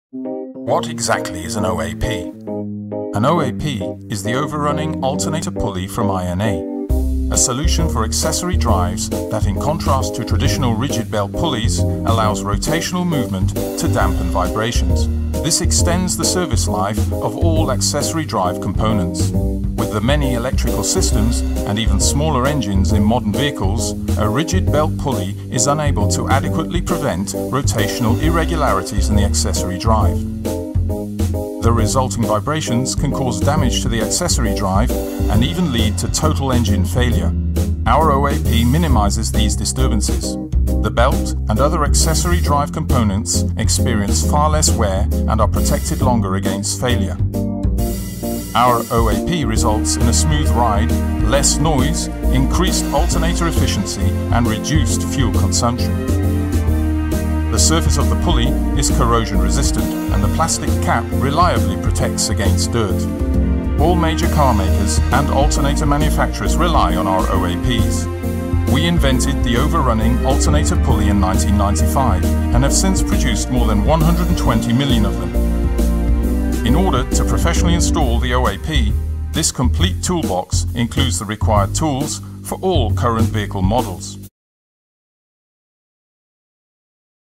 The INA Overrunning Alternator Pulley (OAP) - Simply Explained!
The INA Overrunning Alternator Pulley (OAP) is an auxiliary drive solution that, in contrast to a solid pulley, allows rotational motion in order to dampen vibrations.